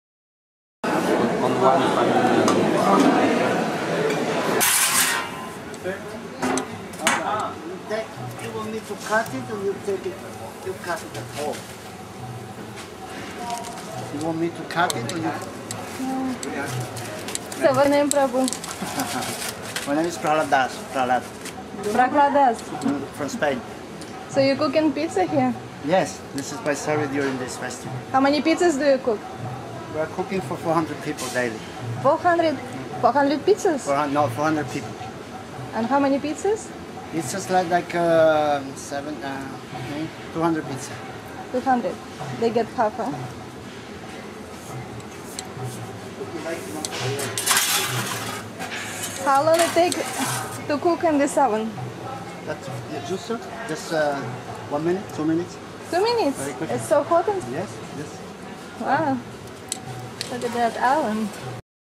Prasad for URI members was cooked by Hare Krishna devotees - pizza
Prasad for URI members was cooked by Hare Krishna devotees, pizza oven was specially built behind brahmachari kitchen. Prahlad Prabhu was cooking 200 pizzas daily for 2 weeks.